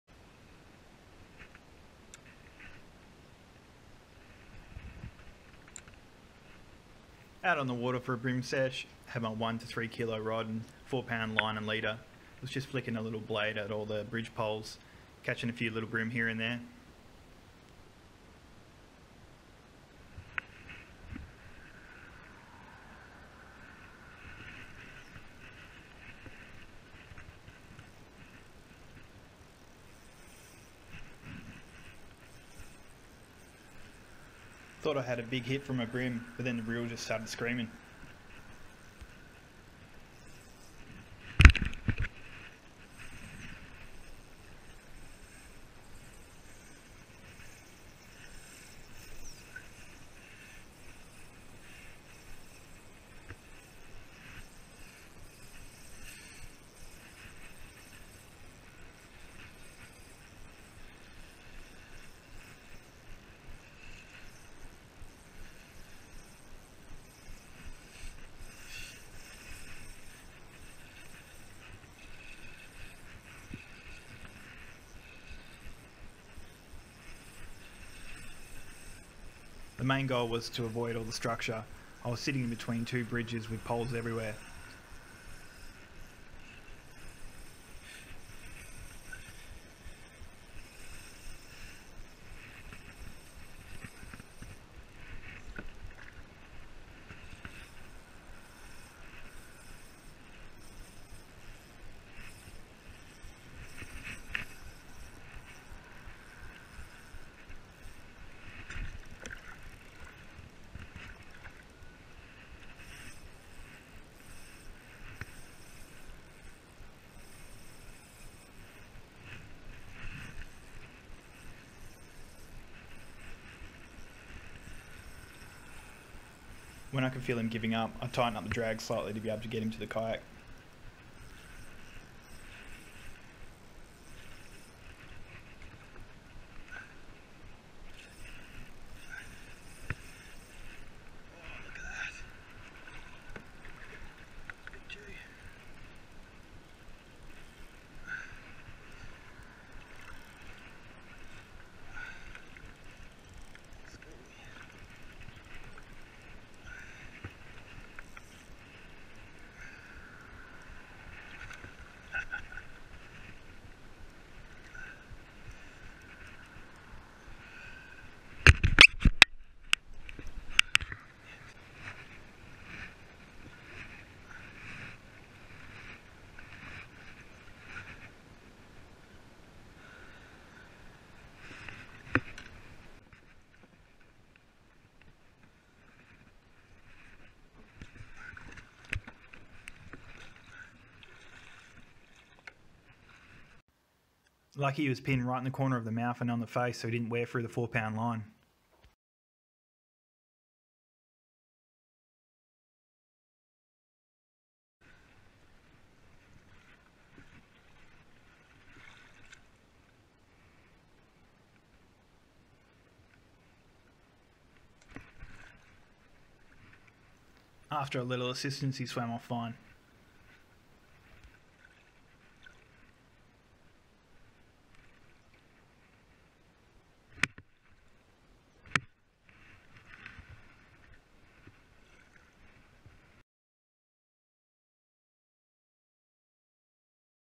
Swan River Upstream - Mulloway /Jewfish - Blade Lure
Caught a School Mulloway on the kayak while chasing bream using 4 pound line and leader. Had to keep him away from the wooden poles and bridge structure on both sides. Gave the little reel a good work out.